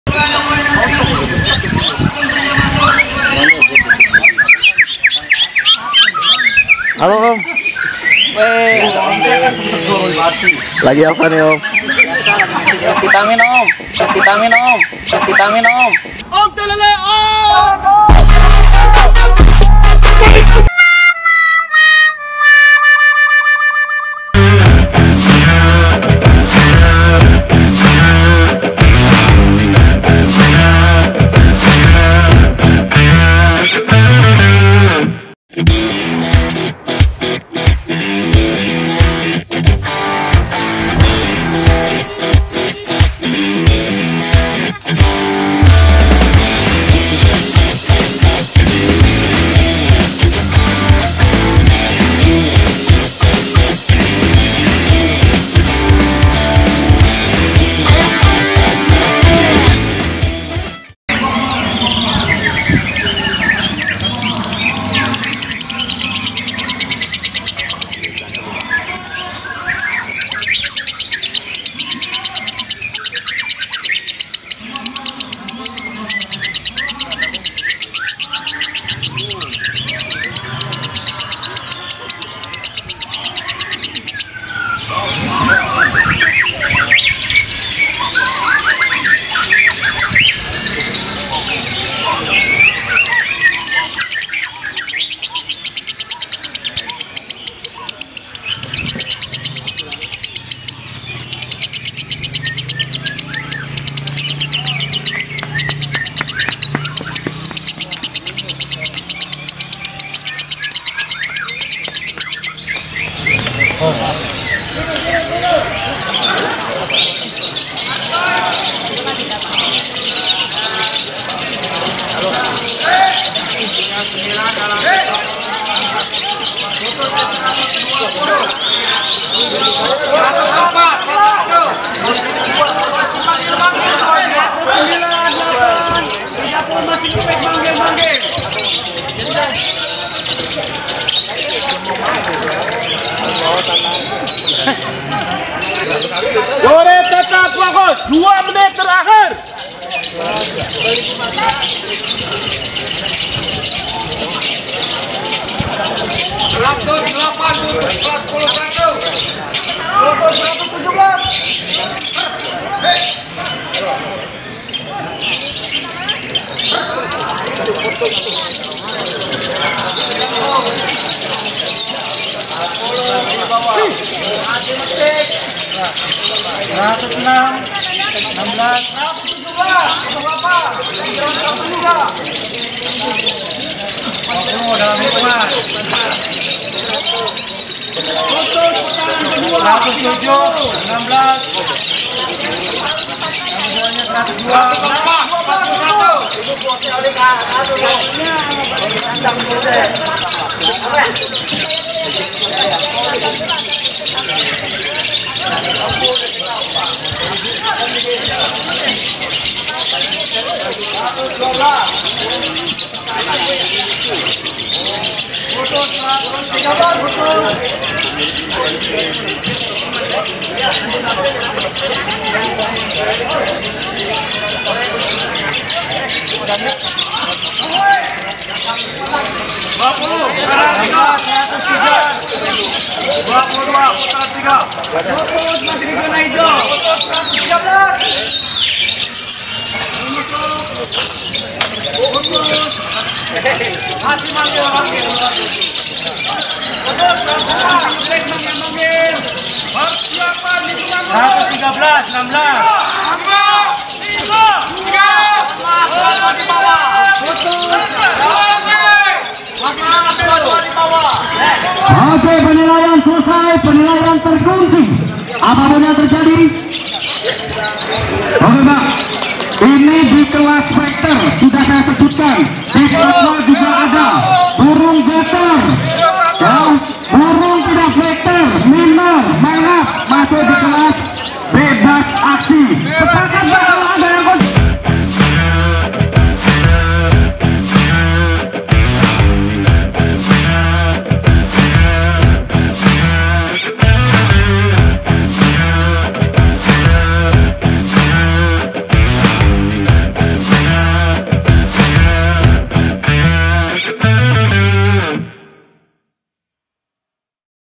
LB CASSANOVA KANAGGA LB HOLIC
LOVEBIRD KONSLET MILIK KANAGGA LB HOLIC PURWAKARTA YANG BARU MUNCUL NAMUN SERING MERAIH PRESTASI.
KITA LIHAT AKSINYA